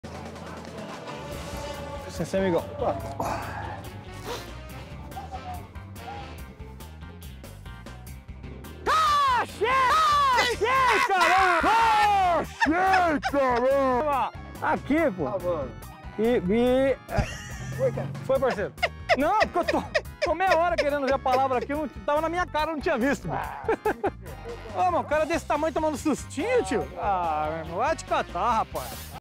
Folgado arruma confusão ao assustar a galera na praça
Rapaz dá um grito em plena praça pública ao acertar a resposta da palavra cruzada e assusta a galera.